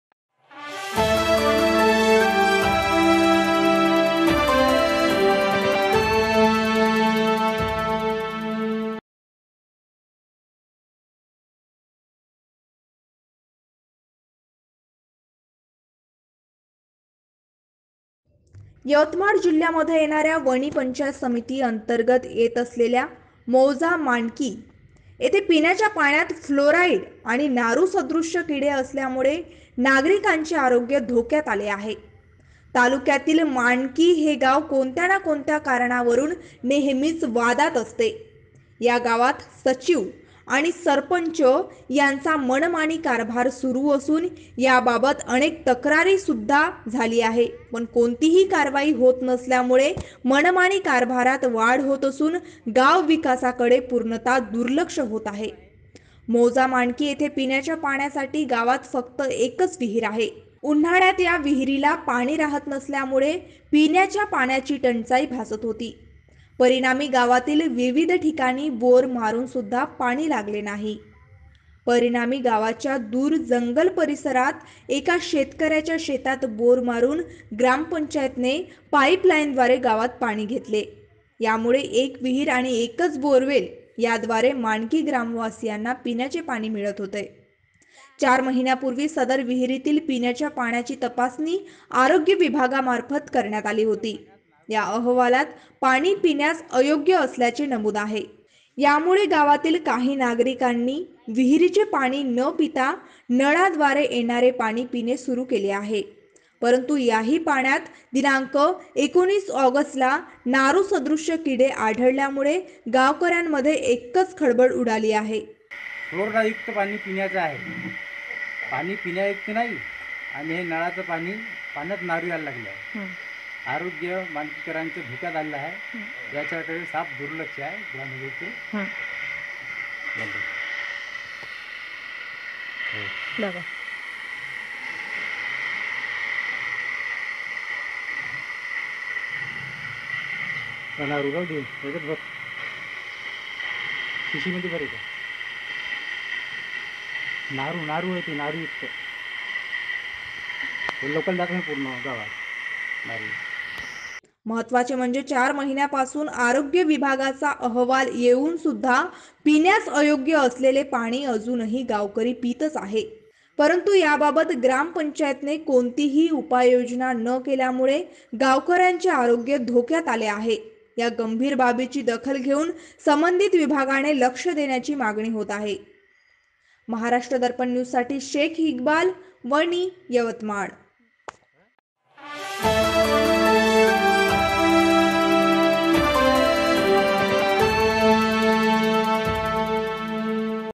यवतमाळ :फ्लोराईडयुक्त व नारुयुक्त पिण्याच्या पाण्यामुळे माणकी ग्रामवासीयांचे आरोग्य धोक्यात
यवतमाळ: फ्लोराईडयुक्त व नारुयुक्त पिण्याच्या पाण्यामुळे माणकी ग्रामवासीयांचे आरोग्य धोक्यात : महाराष्ट्र दर्पण : २२/८/२०१९